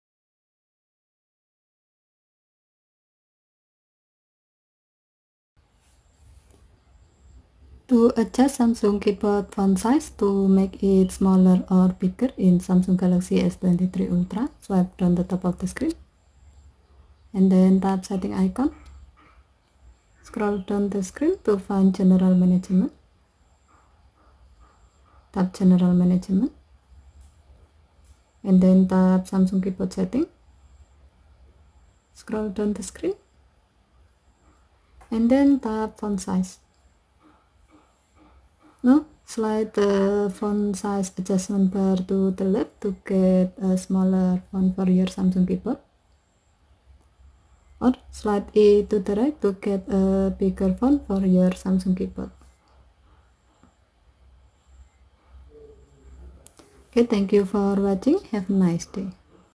Samsung Galaxy S23 Ultra : How to adjust Samsung keyboard font size to make it smaller or bigger
In this video, you will find the steps on How to adjust Samsung keyboard font size to make it smaller or bigger in Samsung Galaxy S23/S23+/S23 Ultra.  This video uses Samsung Galaxy S22 Ultra SM-S918B/DS International version runs with Android 13 (Tiramisu)